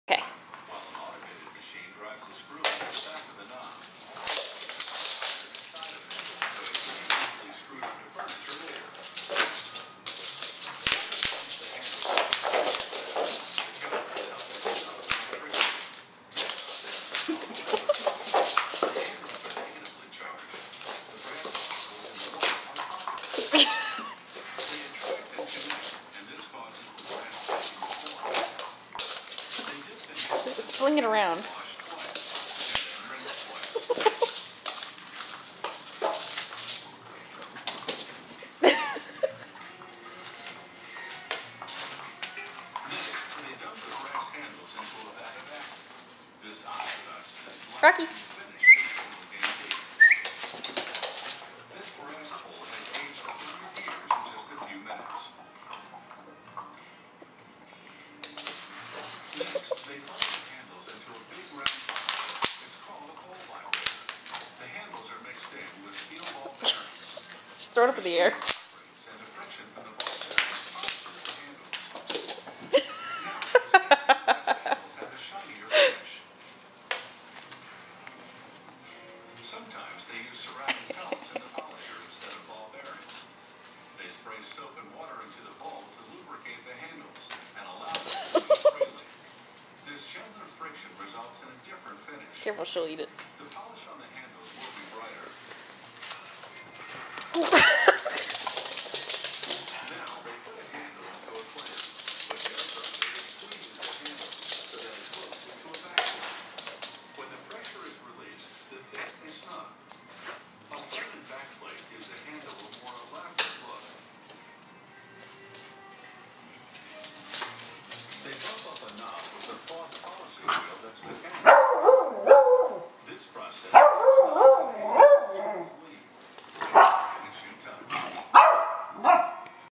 Rocky and the Bubble Wrap
Rocky playing with some bubble wrap.  He doesn't get too excited about many things, so it was nice to see him excited